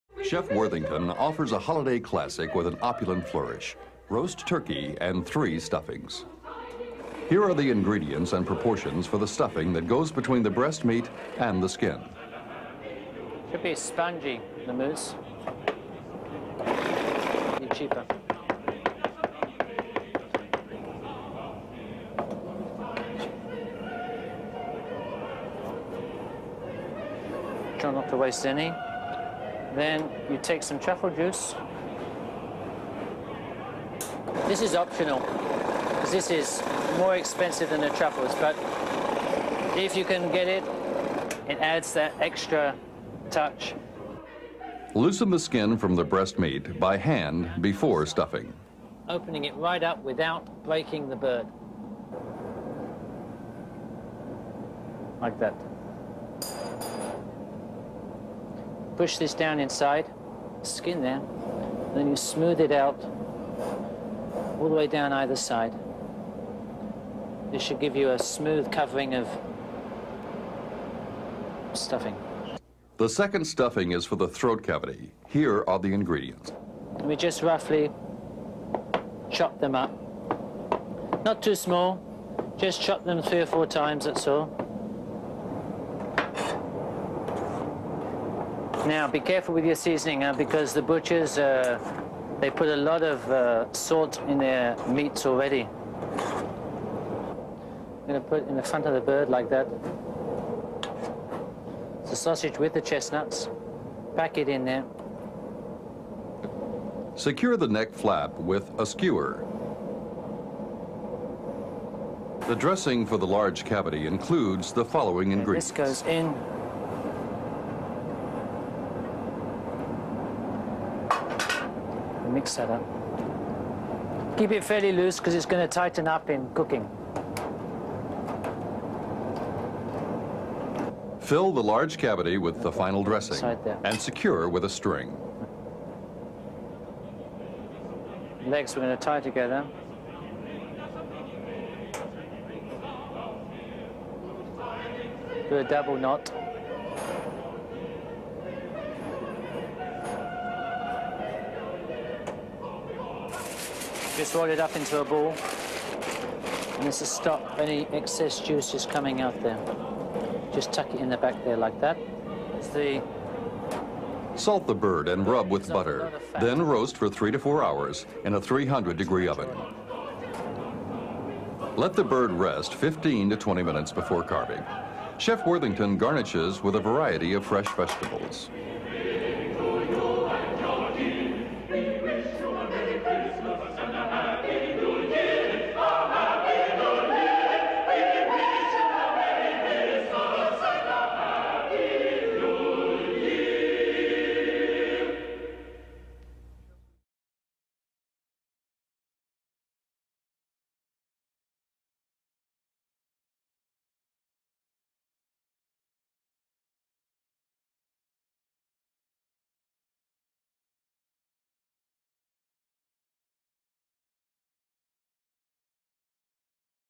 Roast Turkey from Chef Richard Worthington
This version of roast turkey celebrates Richard Worthington's British roots. Three stuffings, two featuring other meats, are used for this quite traditional feast.